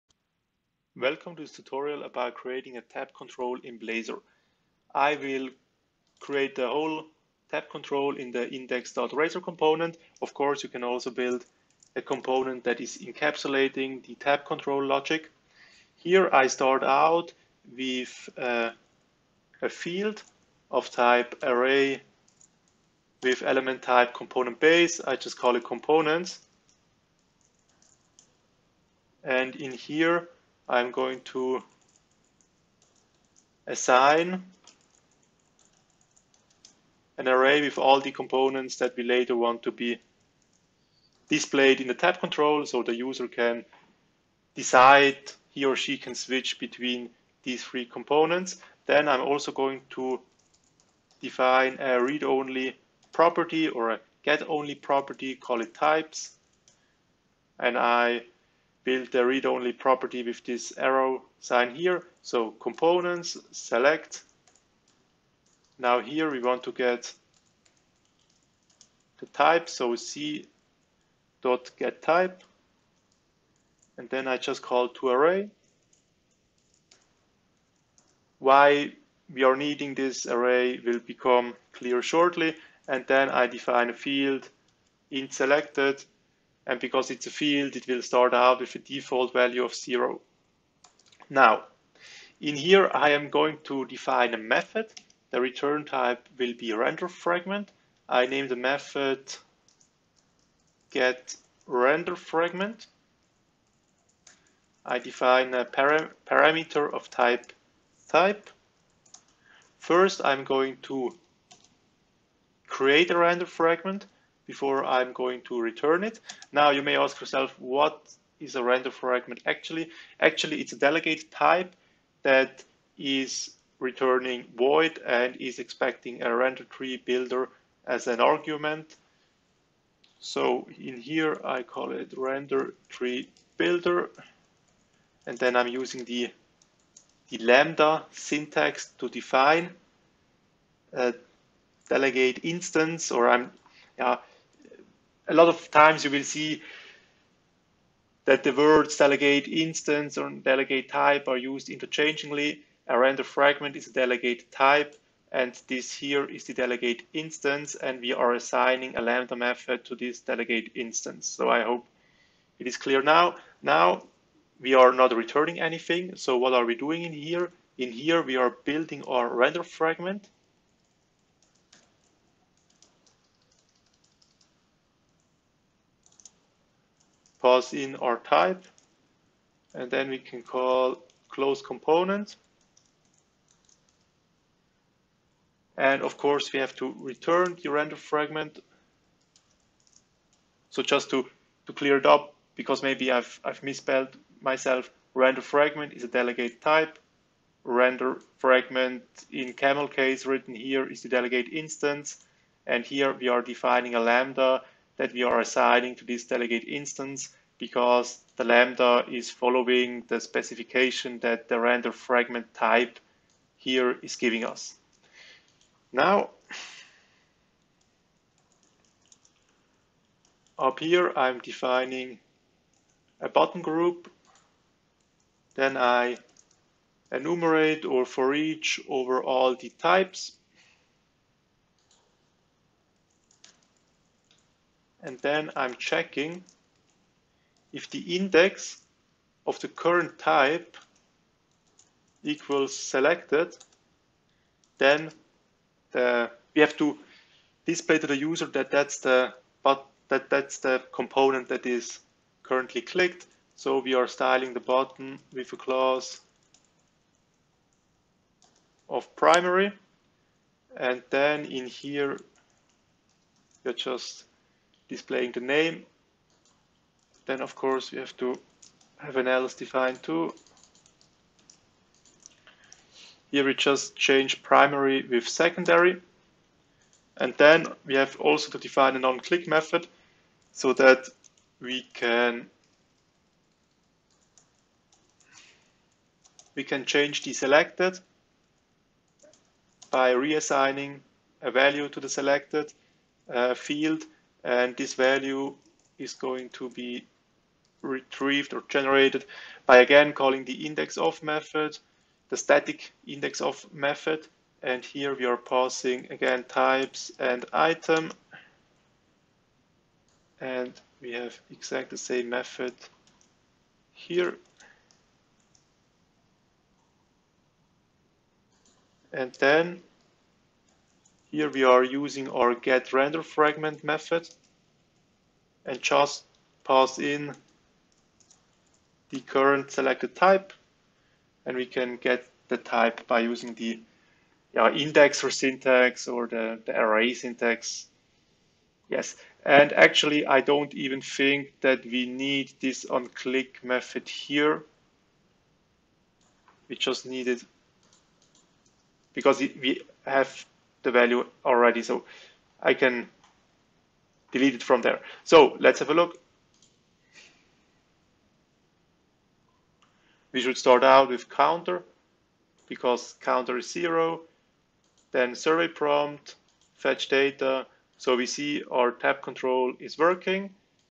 Blazor Tab-Control
In this video I show you how we can build a Tab-Control in Blazor by using the RenderFragment Delegate Type. Code Creator: David Eggenberger